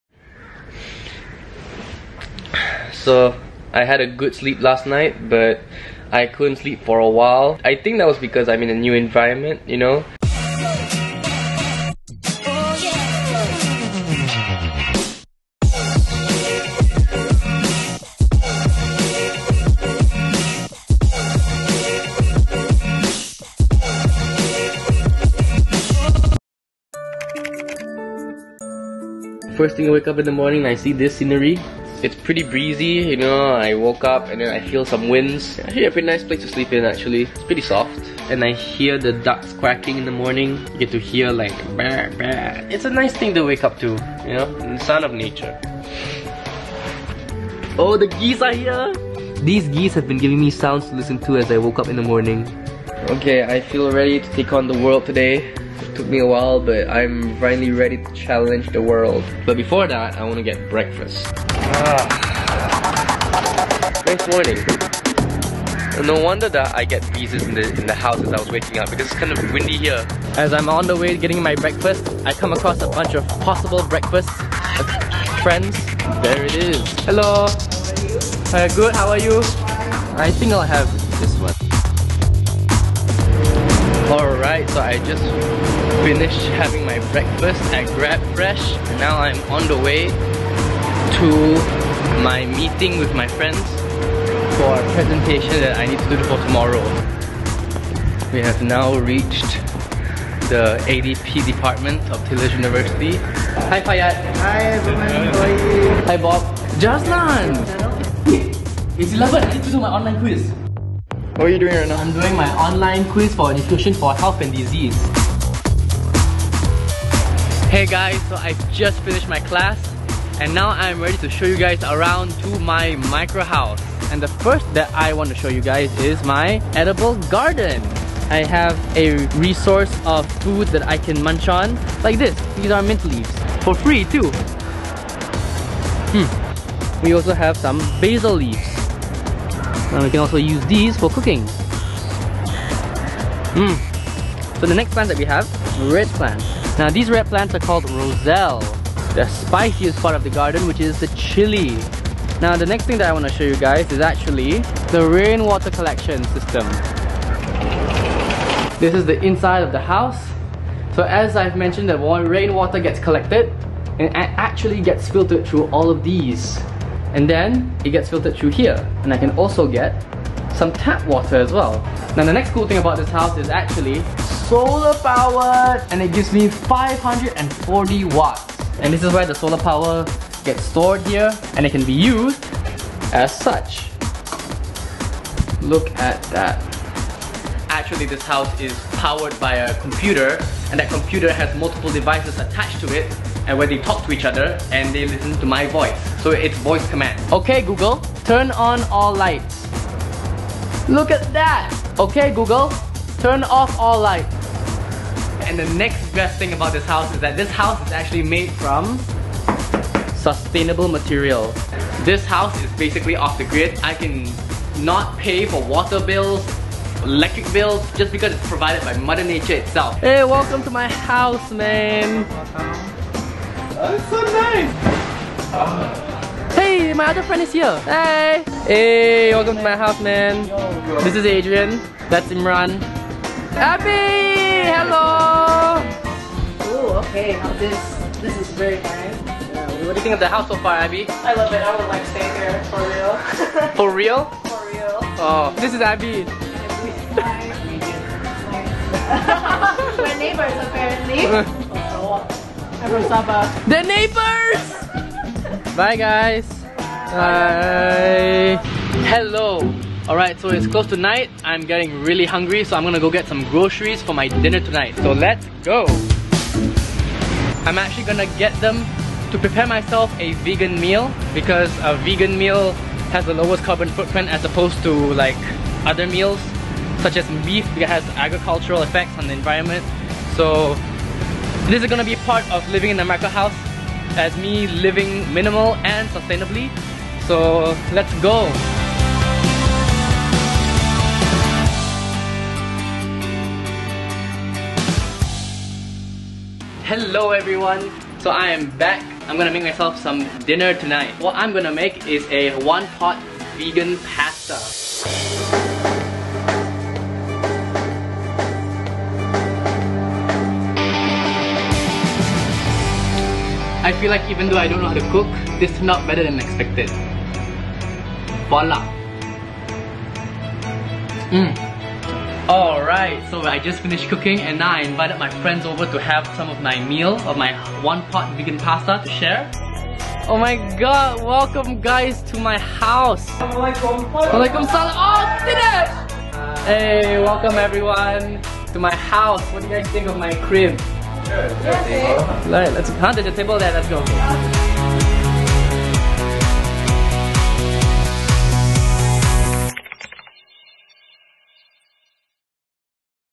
Settling In
Danniel discovers the features of the microhouse and cooks up a one-pot vegan pasta.